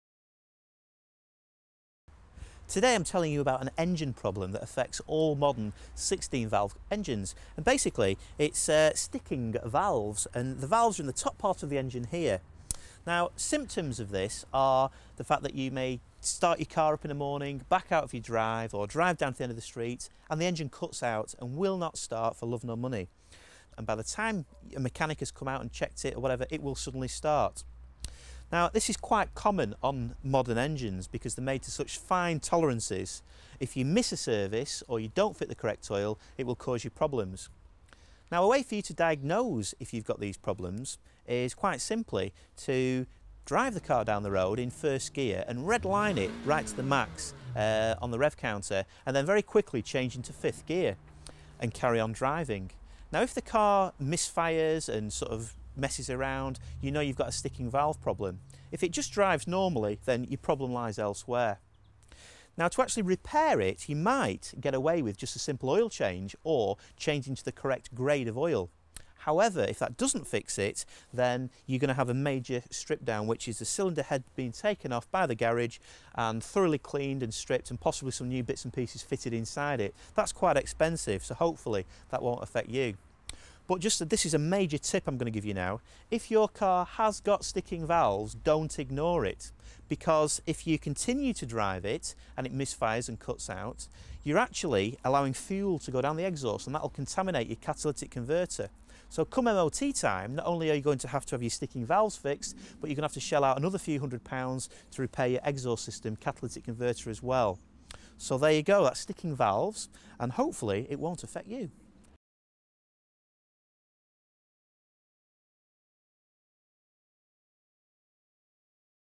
Problems With 16-Valve Engines - How To Fix & Avoid
Simon Hughes talks about a common problem that if faced with modern 16-valve engines, and gives his advice on how to fix the relatively easy-to-fix problem, as well as giving tips on ways to avoid this happening in the first place.